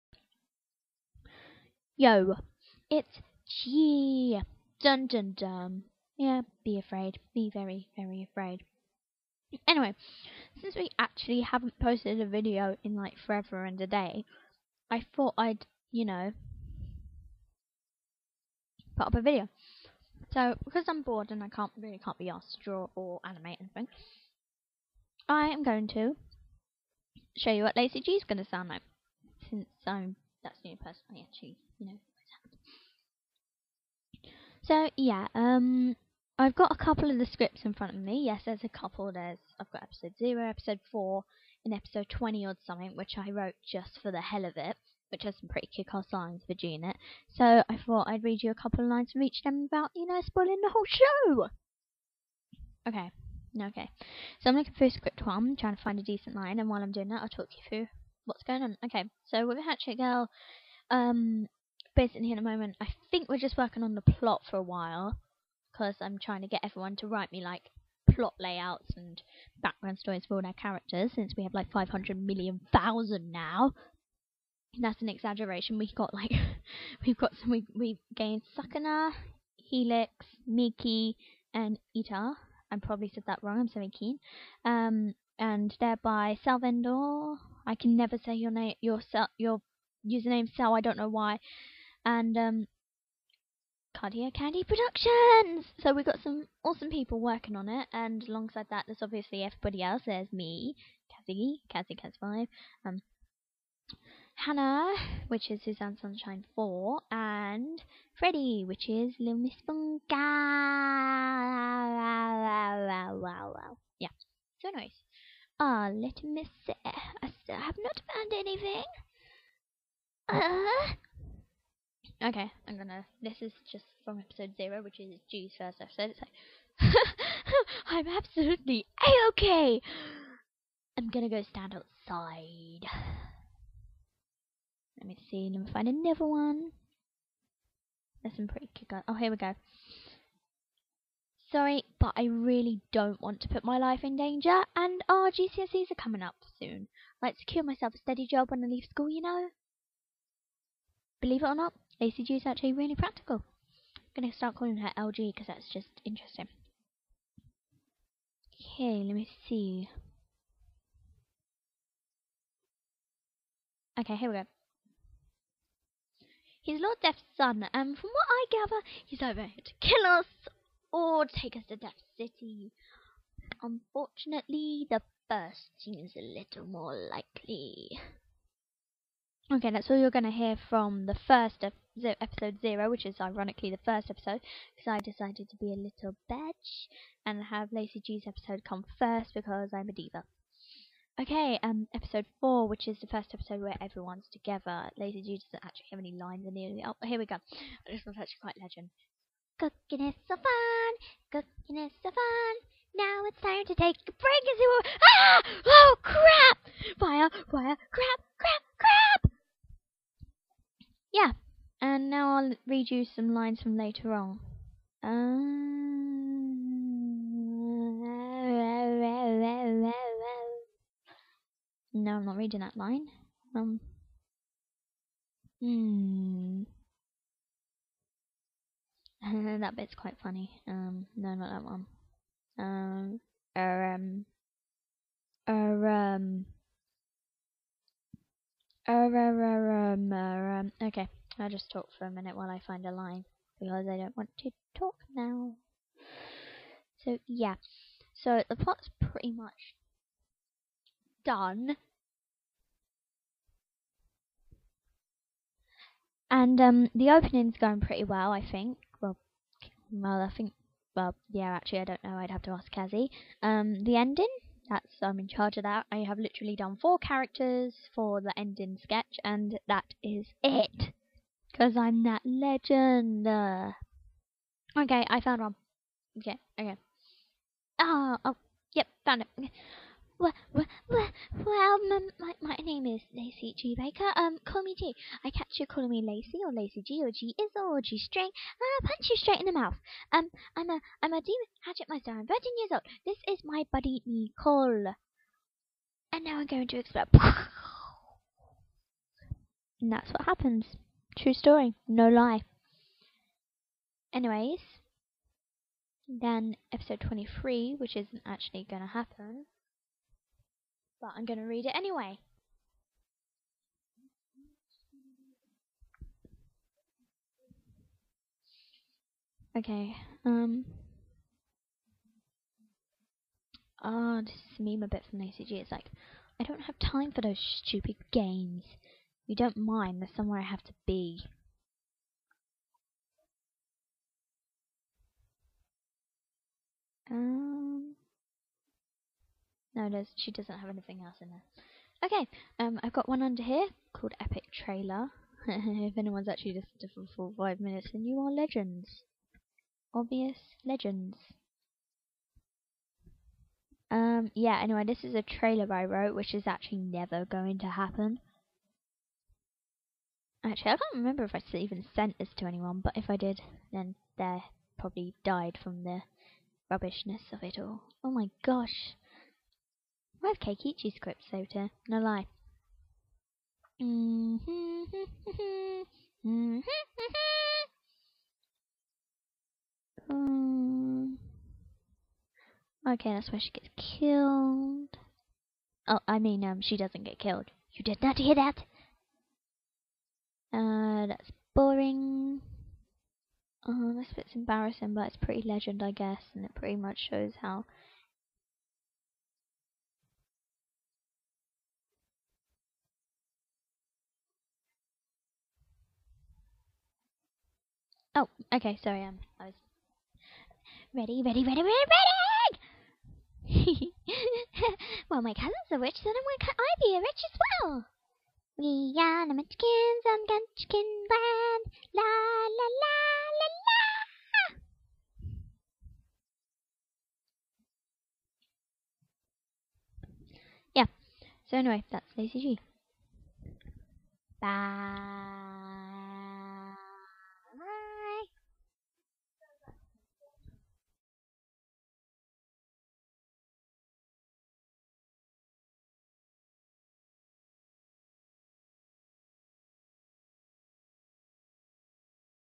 7 Minutes of Lacey-Gee [envyhairday]
7 minutes of me talking about Hatchet Girl and Lacey-Gee. What more could you want?

Art by TornUpHearts Who is a legendary legend and my amazing partner for the Soul Eater OC Tourney!